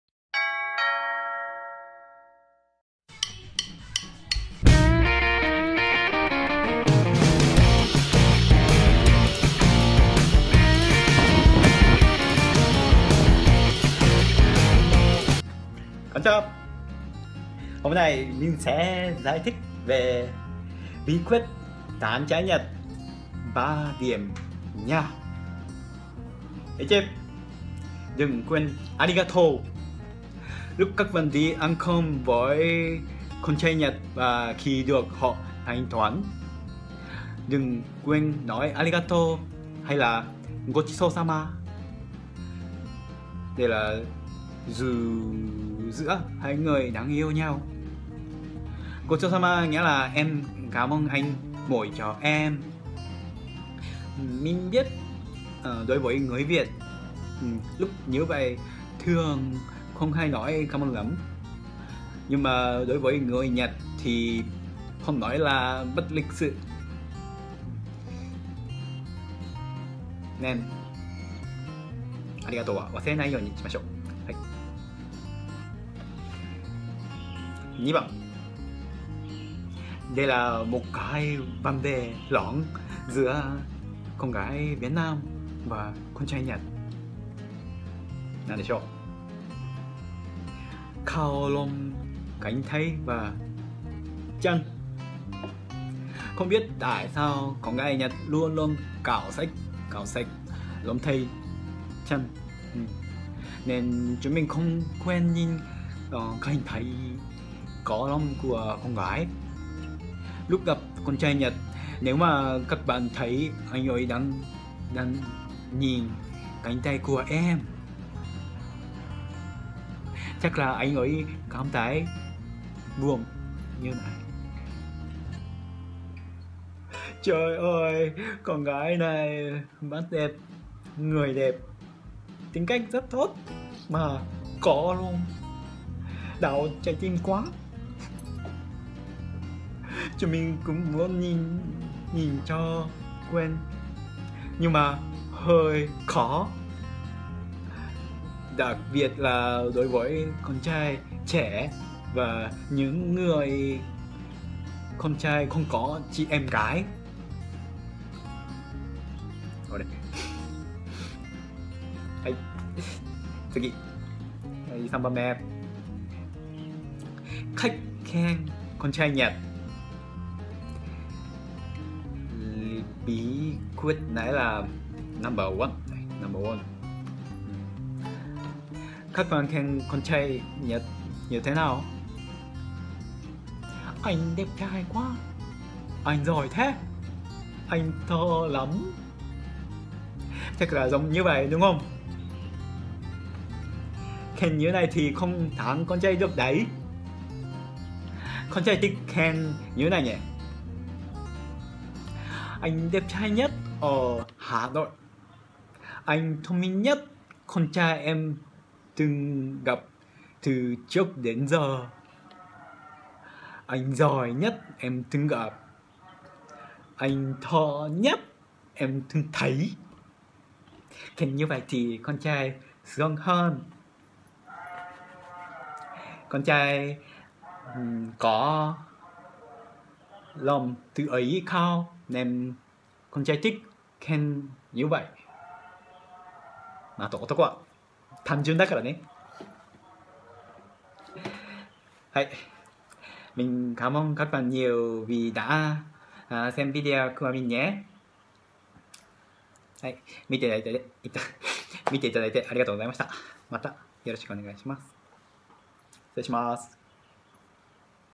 3 lưu ý nhỏ nhỏ đủ để tán trai Nhật.(Bật phụ đề tiếng Việt)
SNS

Mình tên là KiKi người Nhật, mình đang sống ở Hà Nội.
Ước mơ của tôi là mở quán Phở.